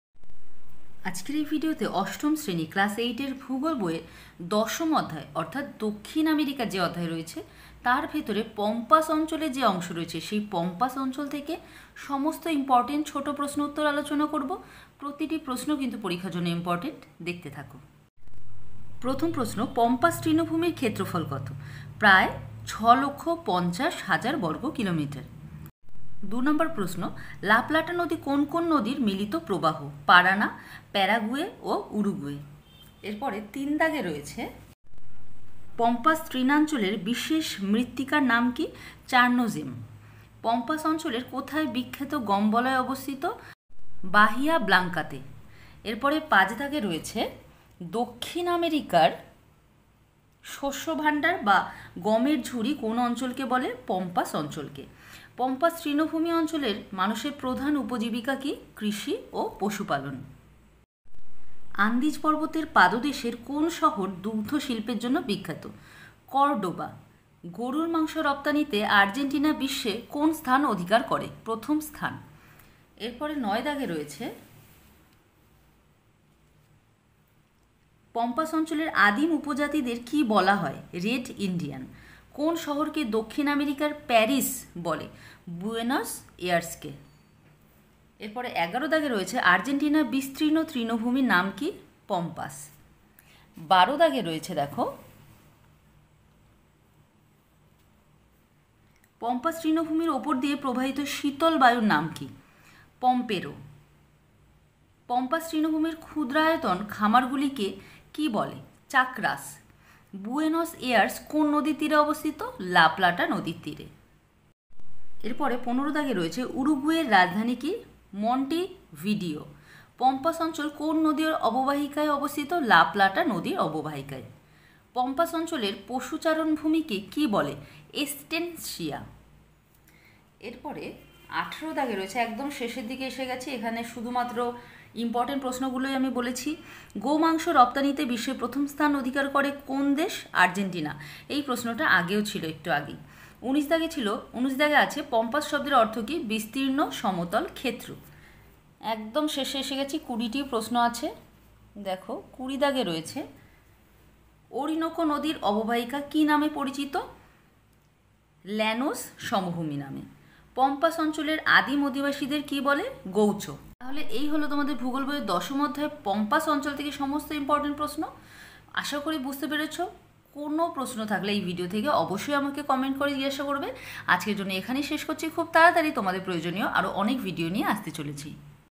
Class 8 Geography/chapter 10/ পম্পাস অঞ্চল/ Short question answer/ WBBSE
অষ্টম শ্রেণীর ভূগোল বই এর দশম অধ্যায় দক্ষিণ আমেরিকার পম্পাস অঞ্চল থেকে ছোট প্রশ্ন উত্তর সম্পর্কে আলোচনা করা হয়েছে।

চ্যানেল এর নাম:
Geography Class with Rakhi Biswas